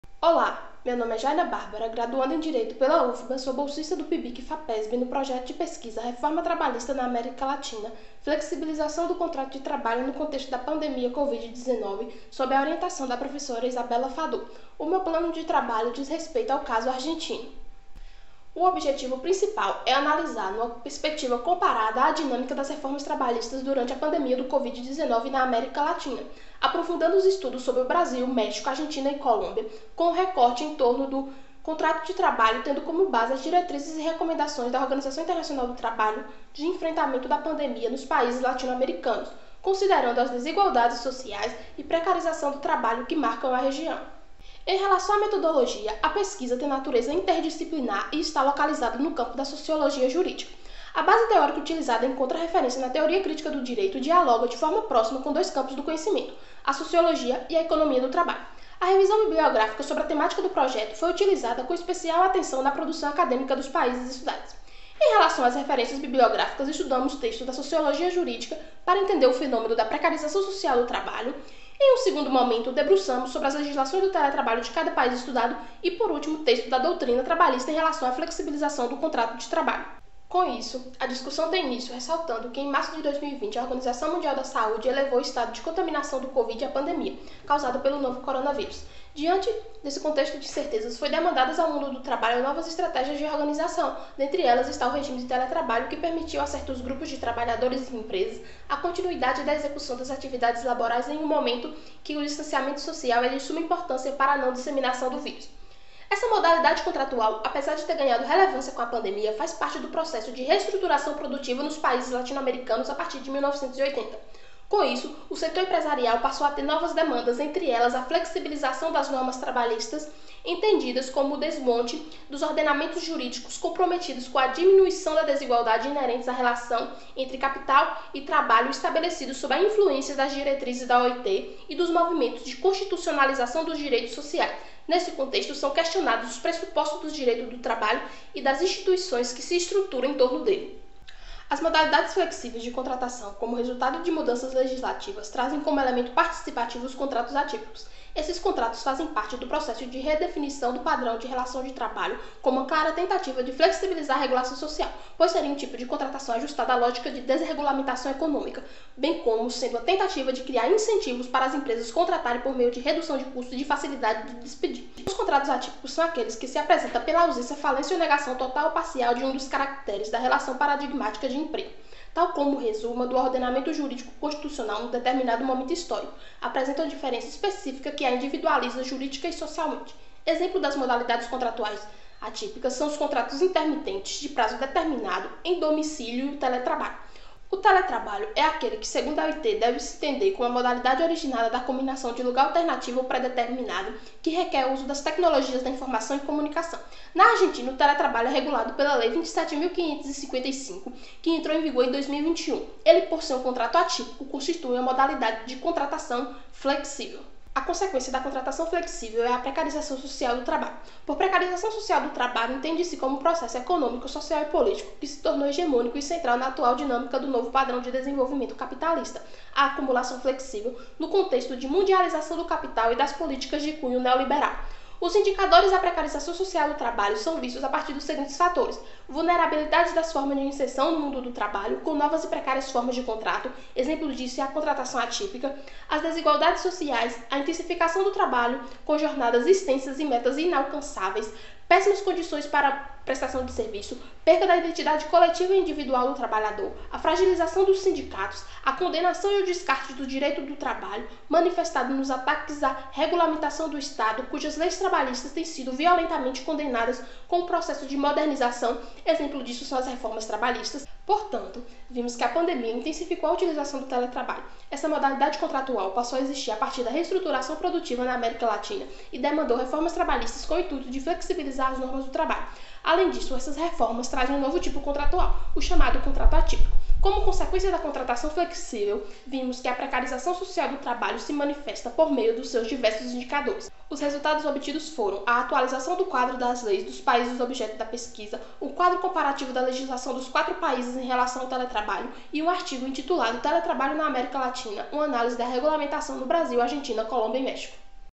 Reforma trabalhista e a regulamentação do contrato de trabalho na Argentina
Reforma trabalhista na América Latina: flexibilização do contrato de trabalho no contexto da Pandemia covid-19.
Autores: Bianca Silva Matos
Ianine Vitória dos Anjos
Jaina Bárbara da Silva
Rosemauro Santos Rocha
A atual conjuntura da Pandemia Covid-19 e a inserção de figuras contratuais atípicas, como o teletrabalho. A forma como esse contrato foi inserido com as flexibilizações das leis trabalhistas ocasionadas pelas Reformas trabalhistas e suas consequências manifestadas no processo de precarização social do trabalho.